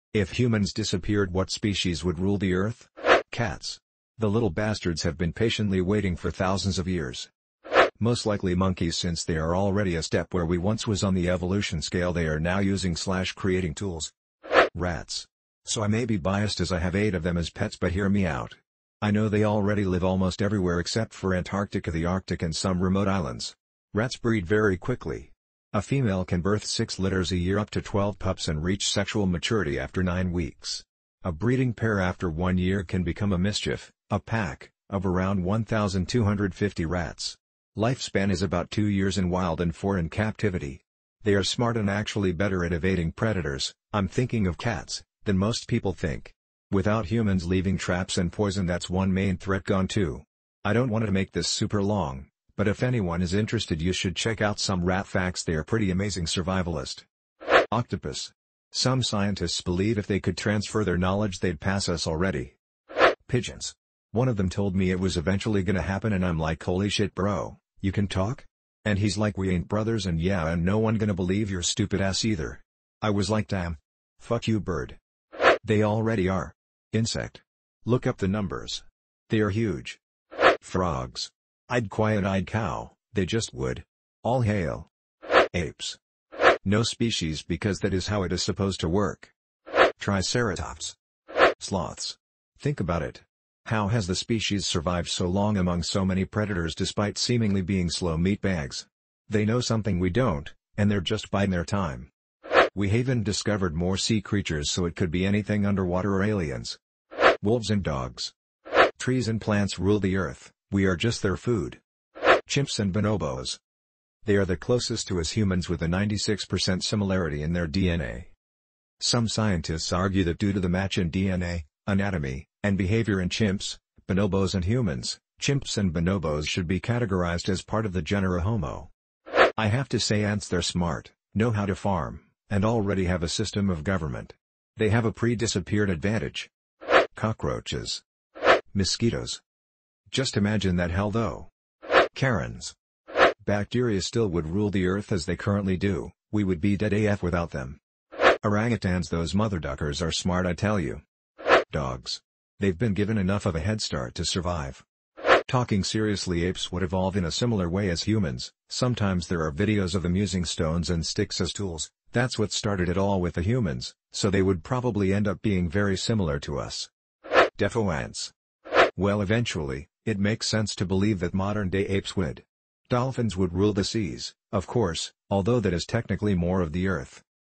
If humans disappeared what species would rule the earth?
If all humans went away what animal would rule the world? Reddit users explain their theories.

Cats. The little bastards have been patiently waiting for thousands of years.

Most likely monkies since they are already a step where we once was on the evolution scale they are now using/creating tools

Ratsâ€¦. So I may be biased as I have 8 of them as pets but hear me out. I know they already live almost everywhere except for Antarctica the Arctic and some remote islands. Rats breed very quickly! A female can birth 6 litters a year up to 12 pups and reach sexual maturity after 9 weeks. A breeding pair after one year can become a mischief (a pack) of around 1,250 rats. Life span is about 2 years in wild and 4 in captivity. They are smart and actually better at evading predators (Iâ€™m thinking of cats) than most people think. Without humans leaving traps and poison thatâ€™s one main threat gone too. I donâ€™t want to make this super long, but if anyone is interested you should check out some rat facts they are pretty amazing survivalist.

Octopus. Some scientists believe if they could transfer their knowledge theyâ€™d pass us already.

Pigeons. One of them told me it was eventually gonna happen and I'm like "Holy shit bro, you can talk?!" And he's like "We ain't bros and yeah and no one gonna believe your stupid ass either." I was like damn.. Fuck you bird

They already are. Insect. Look up the numbers. They are huge.

Frogs. Idk y and Idk how, they just would. All hail.

Apes

No species because that is how it is supposed to work

Triceratops

Sloths. Think about it. How has the species survived so long among so many predators despite seemingly being slow meat bags. They know something we don't, and they're just bidng their time.

We havent discovered more sea creatures so it could be anything underwater or aliens

Wolves and dogs

Trees and plants rule the earth, we are just their food.

Chimps and Bonobos.
They are the closest to us humans with a 96% similarity in their DNA.
Some scientists argue that due to the match in DNA, anatomy, and behaviour in chimps, bonobos and humans, chimps and bonobos should be categorized as part of the genera Homo.

I have to say ants their smart, know how to farm, and all ready have a system of government. They have a pre-disappeared advantage.

Cockroaches

Mosquitoes
Just imagine that hell though

Karens

Bacteria still would rule the Earth as they currently do, we would be dead af without them

Orangutans those motherduckers are smart I tell you

Dogs. They've been given enough of a headstart to survive.

Talking seriously apes would evolve in a similar way as humans, sometimes there are videos of them using stones and sticks as tools, that's what started it all with the humans, so they would probably end up being very similar to us

Deffo Ants

Well eventually, it makes sense to believe that modern day apes would. Dolphins would rule the seas, of course, although that is technically more of the earth